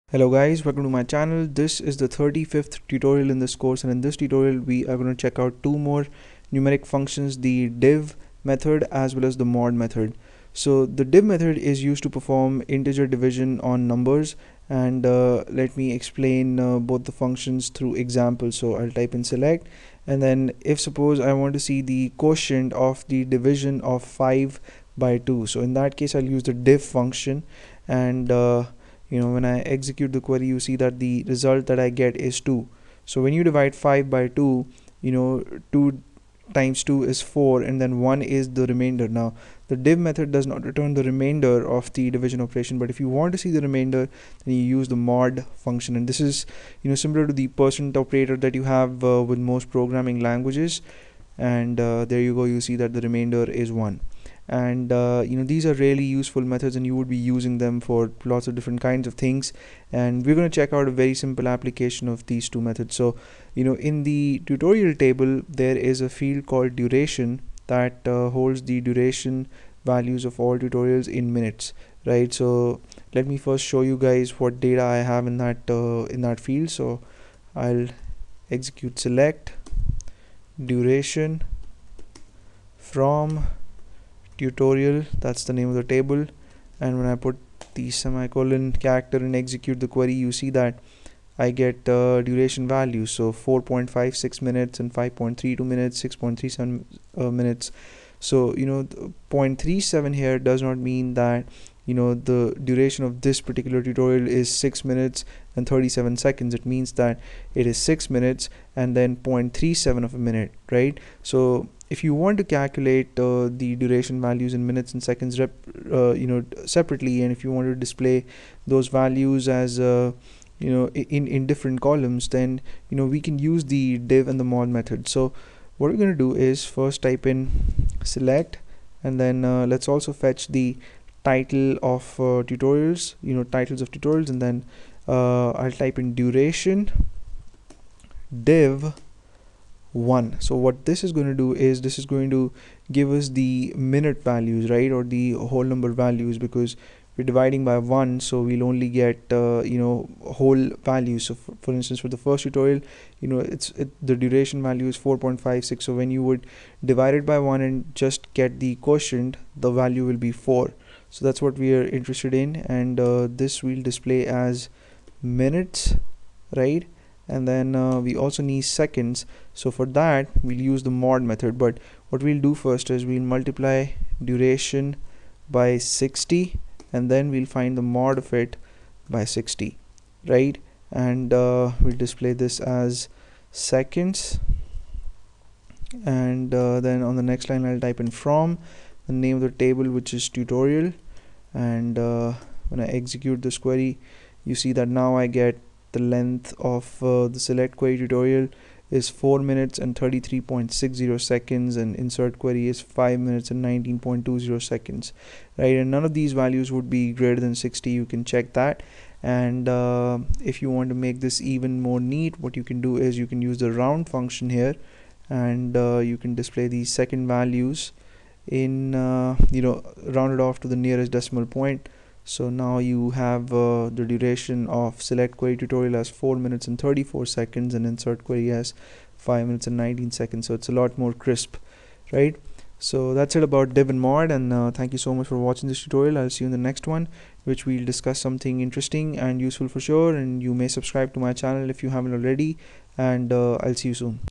SQL Tutorial - 35: DIV() & MOD() Functions
In this tutorial we'll learn to use the DIV() and MOD() functions to obtain the quotient and remainder of division operations respectively.